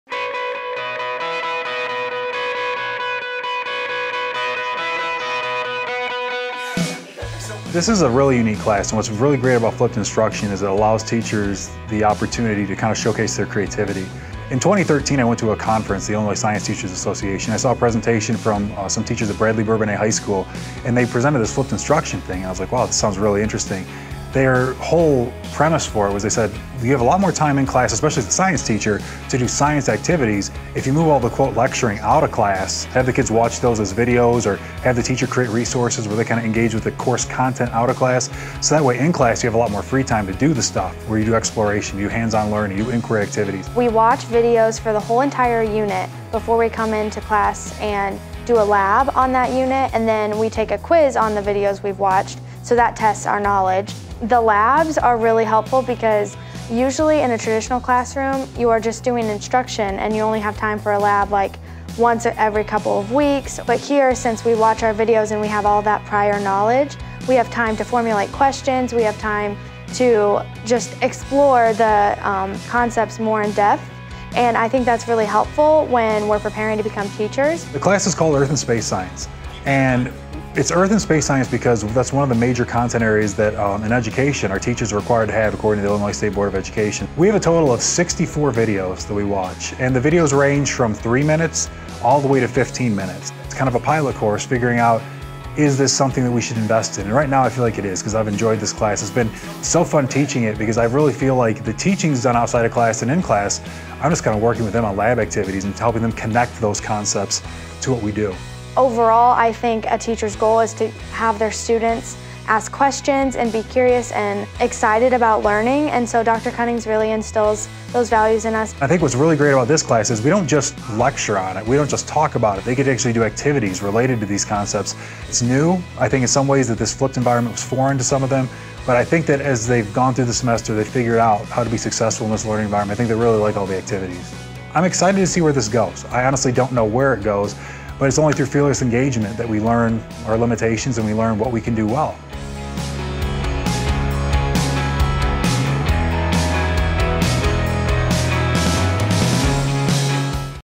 Flipping the Classroom: Earth & Space Science at Millikin University
The term flipped classroom is becoming a unique form of learning and it's being adopted at Millikin University through a pilot course called Earth & Space Science. One Millikin professor is using the flipped classroom concept to have his students watch video podcasts on lessons and subjects outside the classroom, and then meet once a week to apply those lessons to lab work. See how the flipped classroom concept is helping students understand the material.